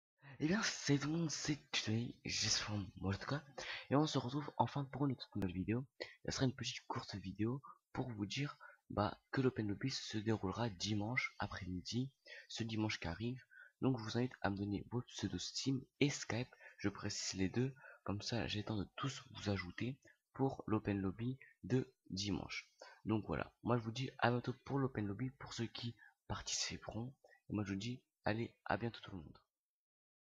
Donnez vos pseudos | Open Lobby Pc
♣ Lisez la descriptions ♣ Commentez ♣ Aimez ♣
Salut tous le monde , On se retrouve pour une vidéo open lobby sur ma chaîne :)
Allez sur tous ceci pour mieux me suivre :
Mon skype : Youtubetutodanny
Mon steam : Kidoboss